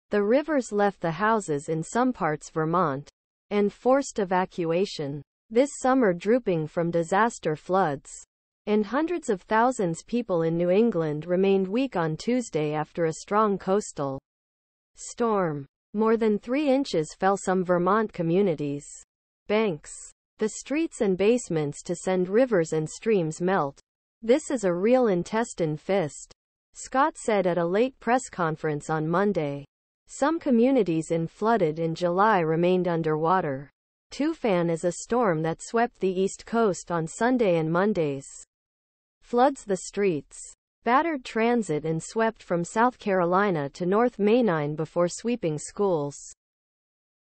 Power Outages and Flooding Afflict New England After Storm
The rivers left the houses in some parts Vermont, and forced evacuation, this summer drooping from disaster floods, and hundreds of thousands people in New England remained weak on Tuesday after a strong coastal storm. More than three inches fell some Vermont communities, banks, the streets and basements to send rivers and streams melt. "This is a real intestine fist," Scott said at a late press conference on Monday, some communities in flooded in July remained underwater. Tufan is a storm that swept the east coast on Sunday and Mondays, floods the streets, battered transit and swept from South Carolina to North Mainine before sweeping schools.